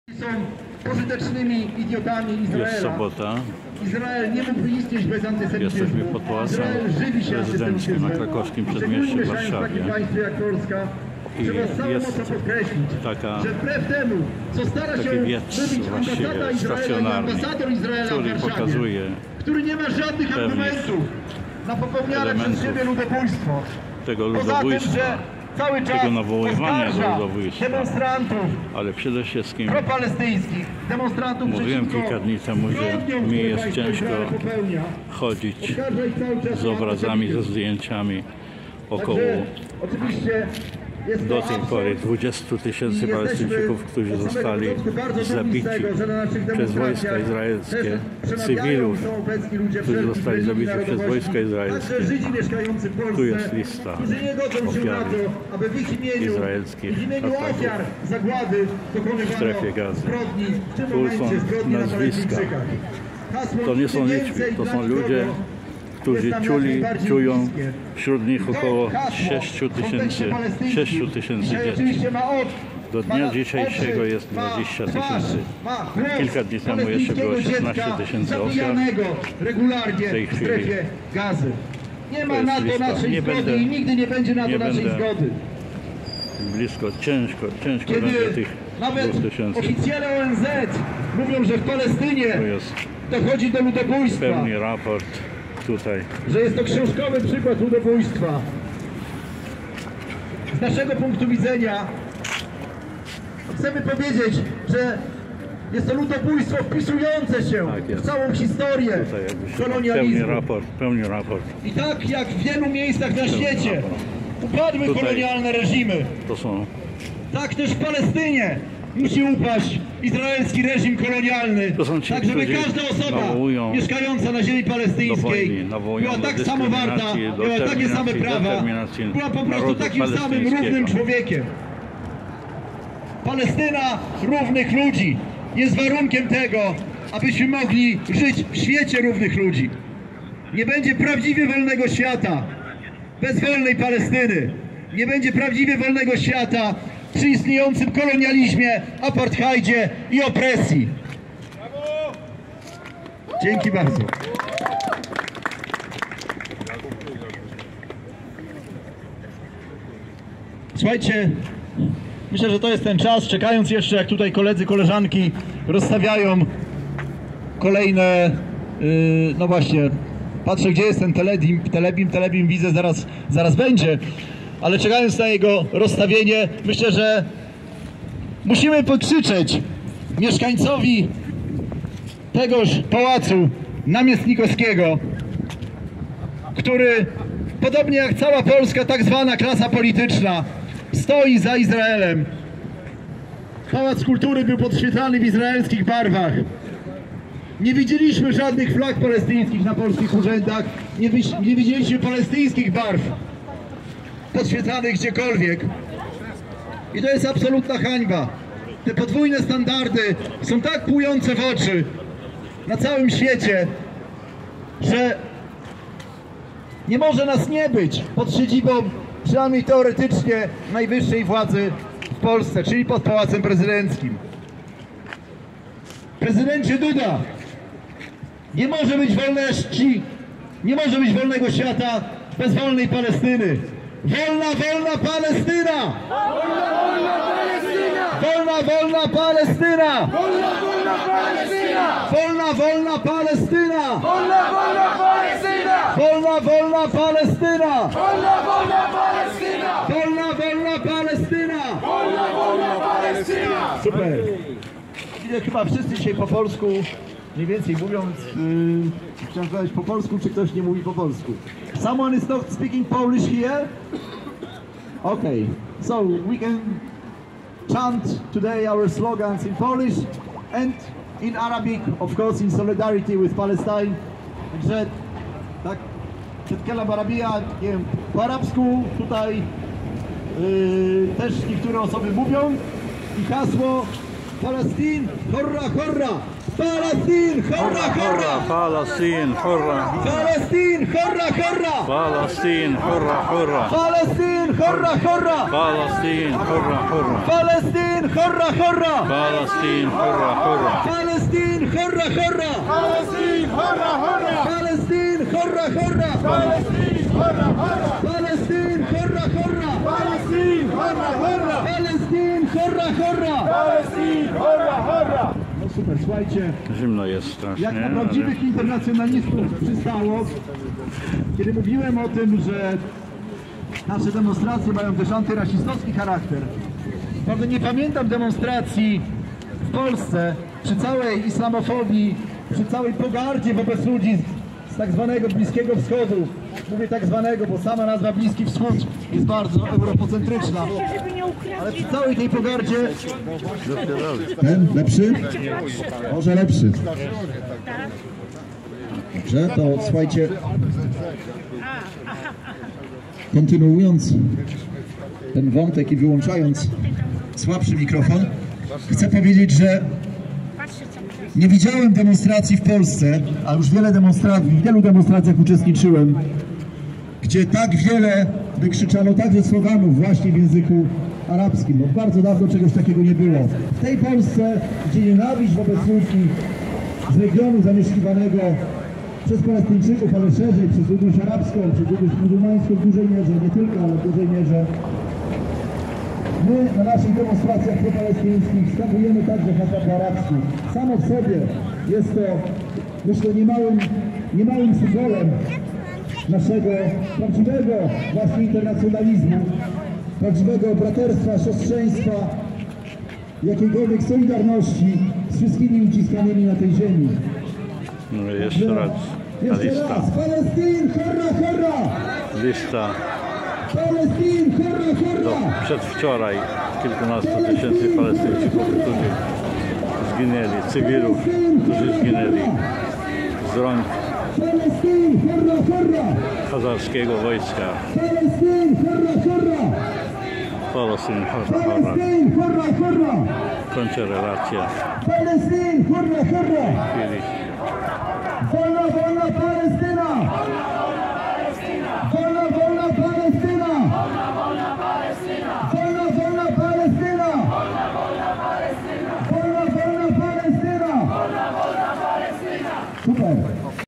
25.11.23 r. - GAZA przeżywamy - Warszawa Pałac Prezydencki - Skrócona lista Poległych Palestyńczyków
Drugi dzień rozejmu i wymiany izraelskich gości Hamasu na Palestyńskich aresztowanych przez wojska okupacyjne Palestyńczyków .
Ostatni bilans ok. 20.000 Poległych Palestyńczyków w tym ponad 6.000 dzieci. 5.000 zaginionych znajdujących się jeszcze pod gruzami.
nr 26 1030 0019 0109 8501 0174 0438 PLN,
Nabil Malazi w CITI HANDLOWY z dopiskiem "DAROWIZNA" .
Lub Zrzutką na:.